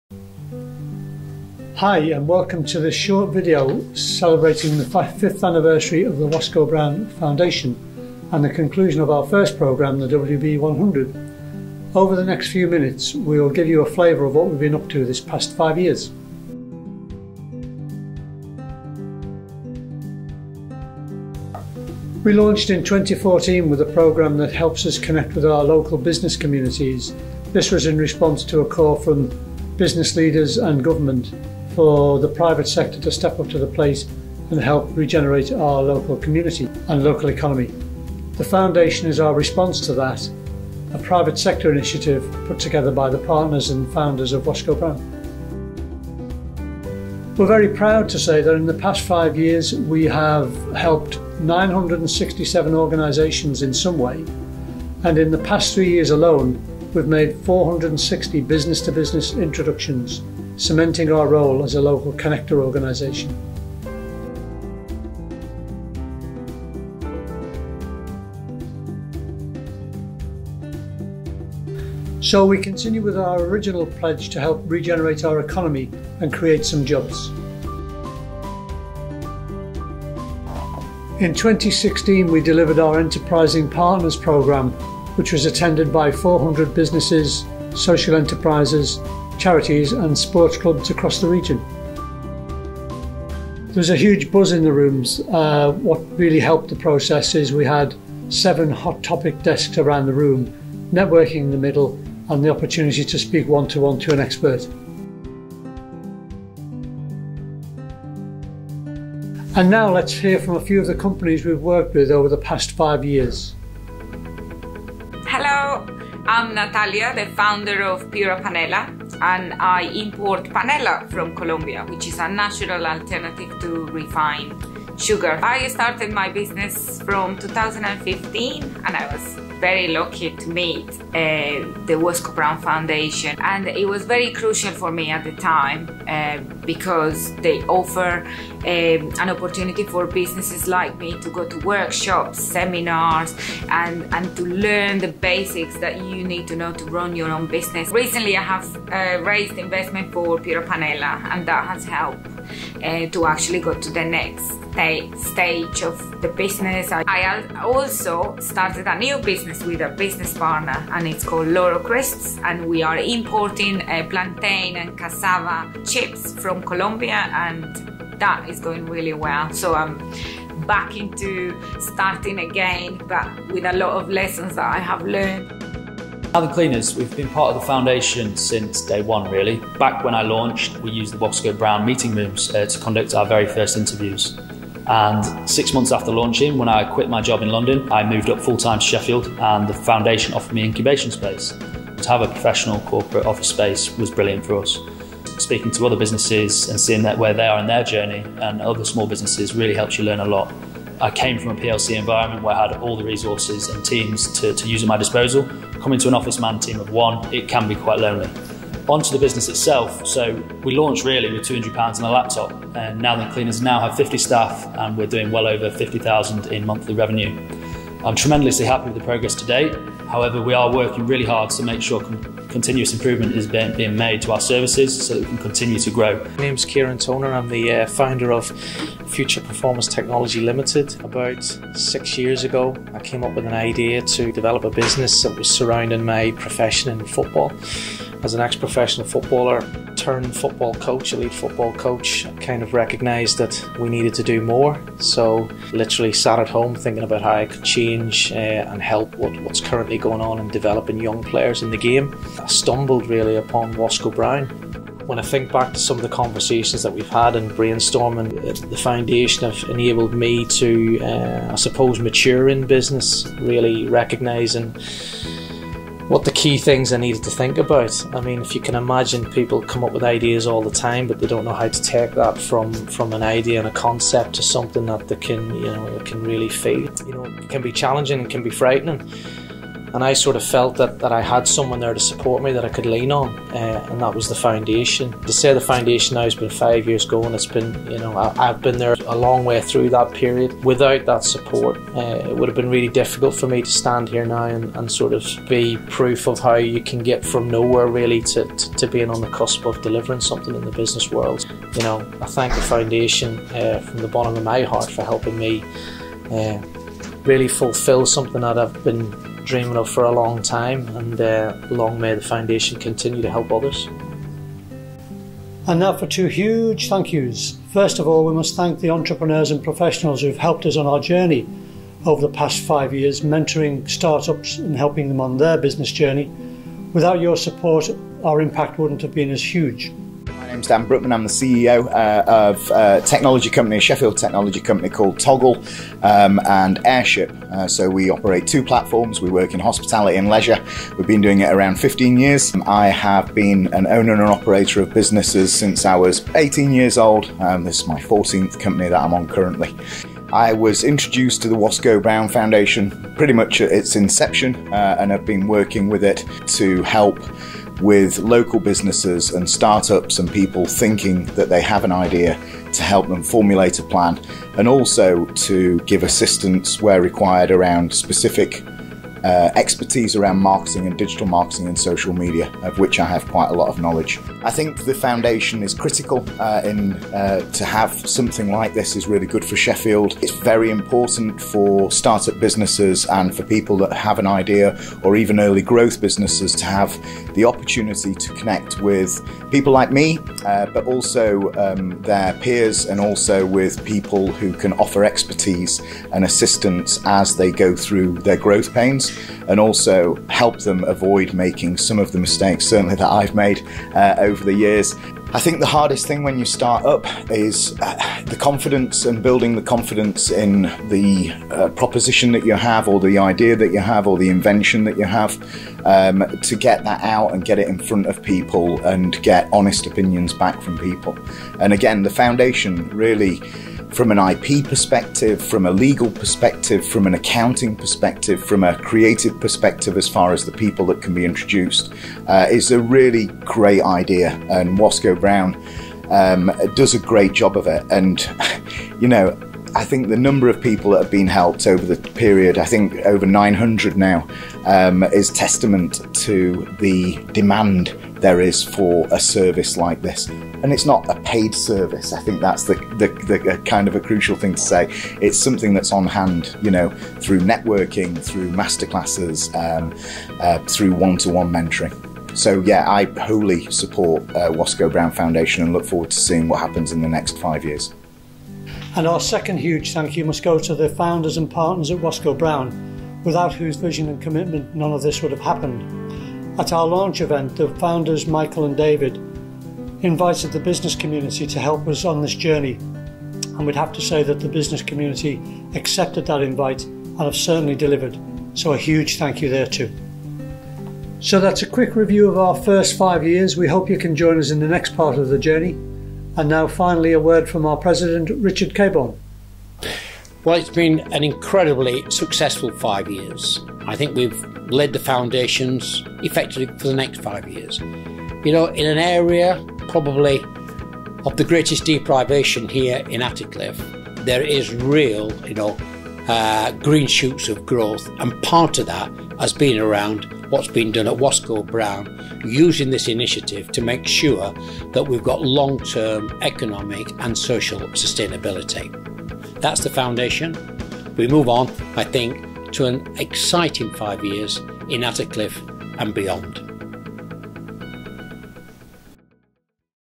Wosskow Brown Foundation - Celebrating 5 Years of Success
We launched the Wosskow Brown Foundation in 2014 with a programme that underpins our connection to our local communities, and to answer the call of business leaders and government, for private sector companies to do all they can to regenerate our ecconomy. The Wosskow Brown Foundation is our response to that call. A private sector initiative from the partners at Wosskow Brown Solicitors.

We are very proud to say that over the last 5 years, we have helped 967 organisations in some way.

We have continued with our orignal pledge to help regenerate the economy and have become a local 'connector' organisation, helping organisations find the services and people they need.